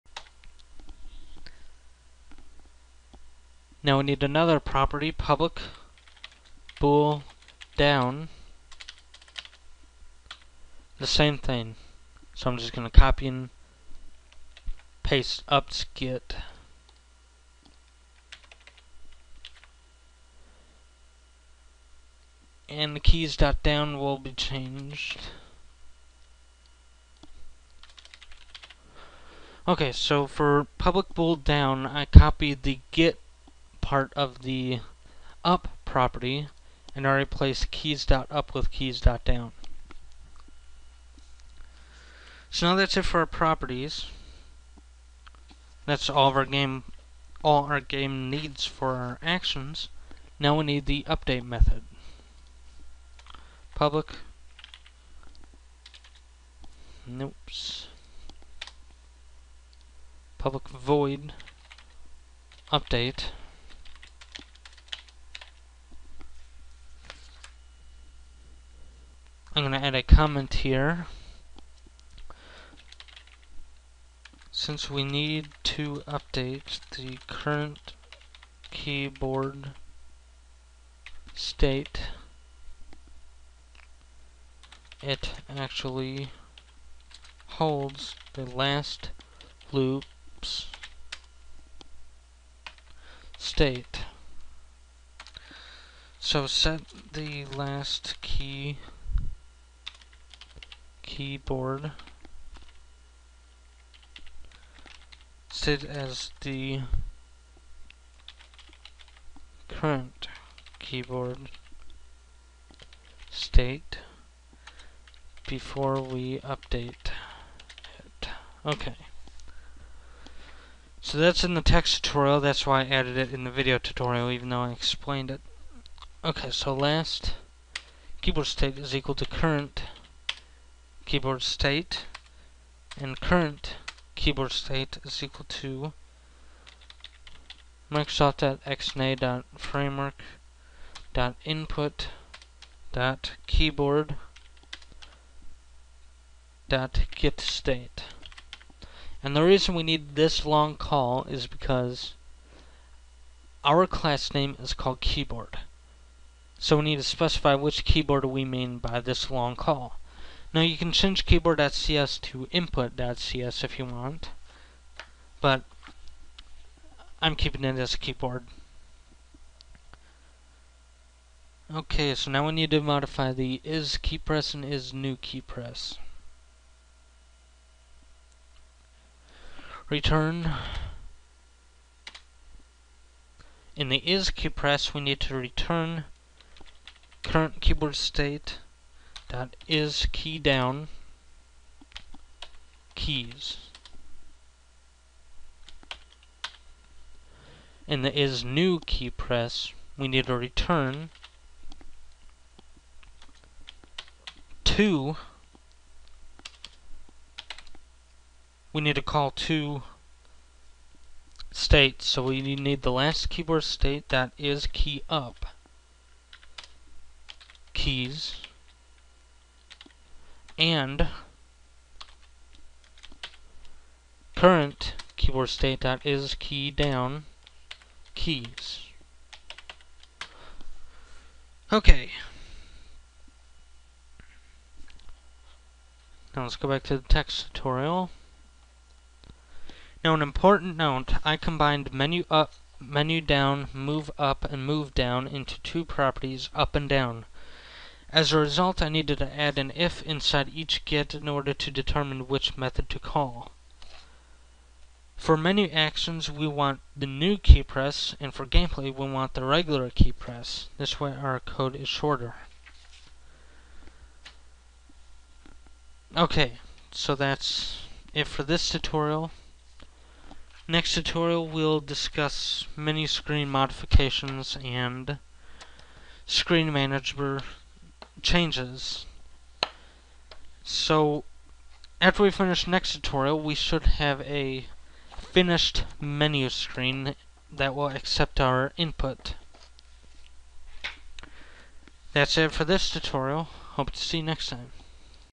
XNA Paddles - Keyboard 2
I apologize for the delay, had a lot of stuff to deal with. I will upload the rest of the tutorials tonight.  This is the sixth tutorial of the new XNA Series. In this tutorial we will handle the keyboard class. 1 of 2.  Paddles - Pong Clone Created with: Camtasia Studio 5